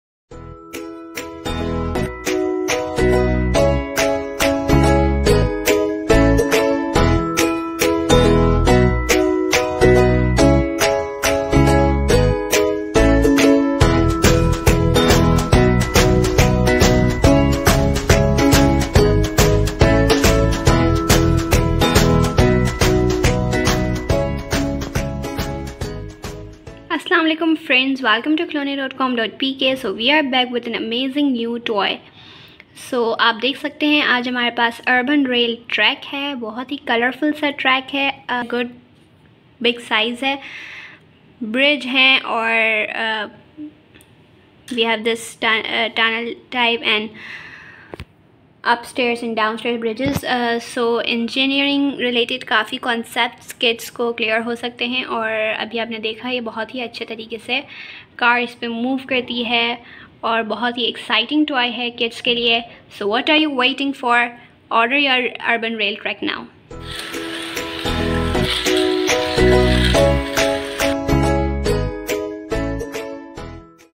Urban Rail Track
High-quality material. Adopting eco-friendly plastic material and electronic components, non toxic, and impact resistance.

Ability training: Crawling, interest cultivation, parent-child communication, auditory, interactive toys.

The car adopts electric universal wheels, it will turn automatically when it meets obstacles.

Material: Plastic & electronic components.